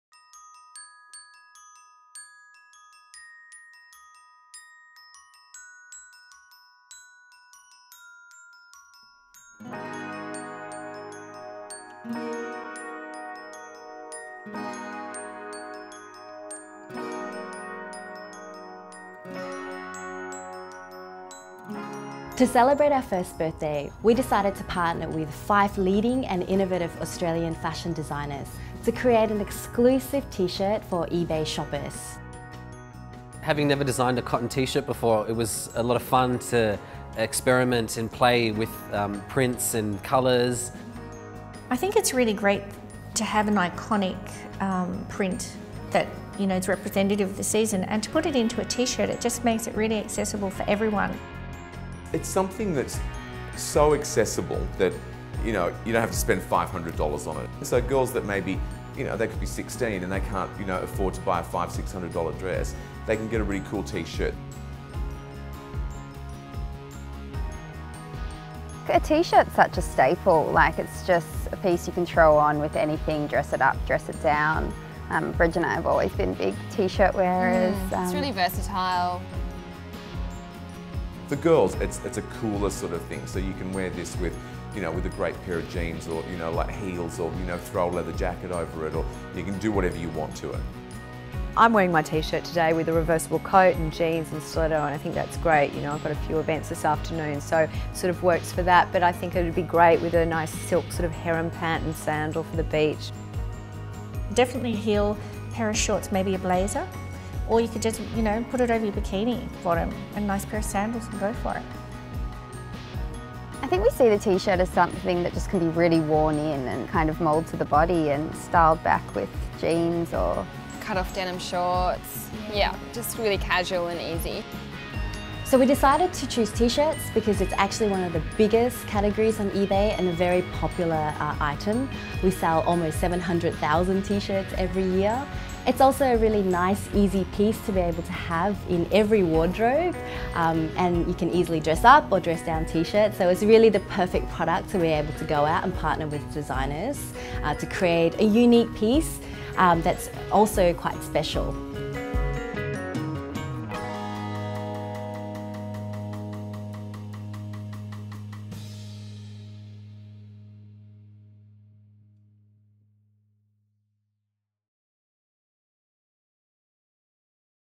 Fashion Gallery at eBay Designer T-Shirts
Fashion Gallery at eBay has partnered up with five of Australia's leading fashion designers Alex Perry, Camilla, Bec & Bridge, Lisa Ho and Michael Lo Sordo to design a limited edition t-shirt, modelled by Lara Bingle. Collect them all NOW